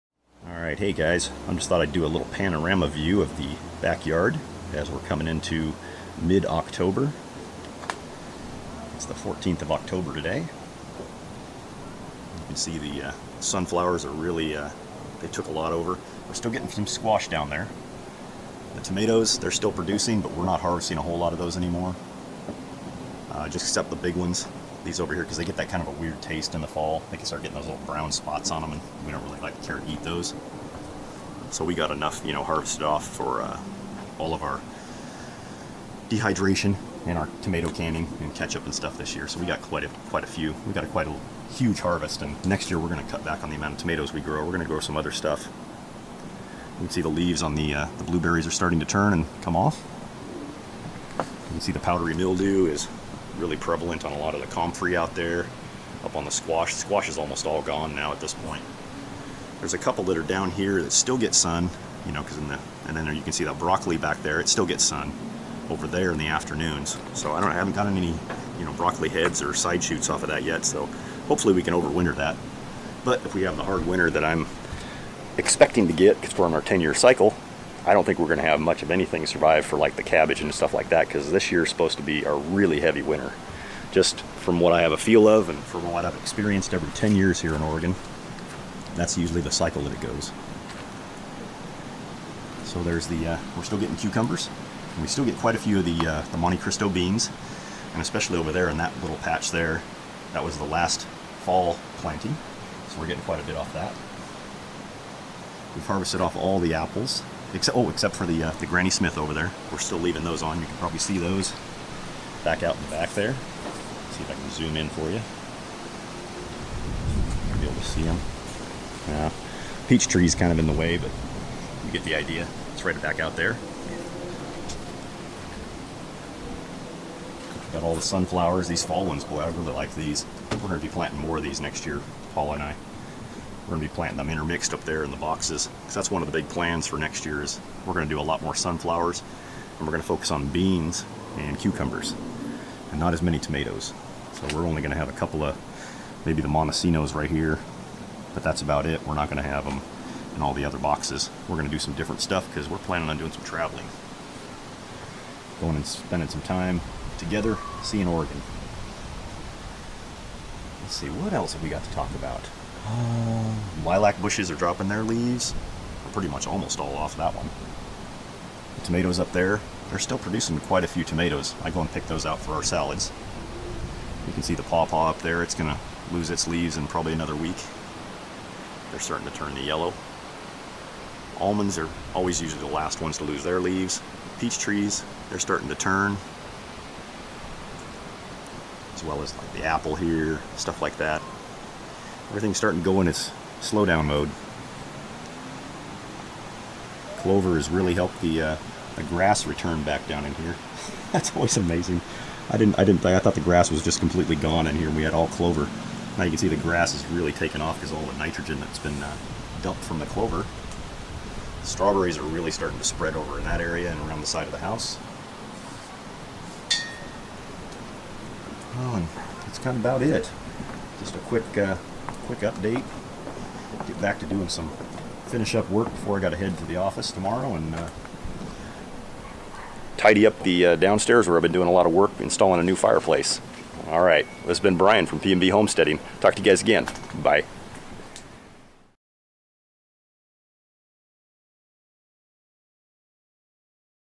Backyard homestead panorama 2018 week 39
Hi Everyone, with the work I've taken on inside I thought a little panorama video would be a nice update. The yard is heading into its fall sleep and the 2018 garden is wrapping up. Enjoy the video update! :-)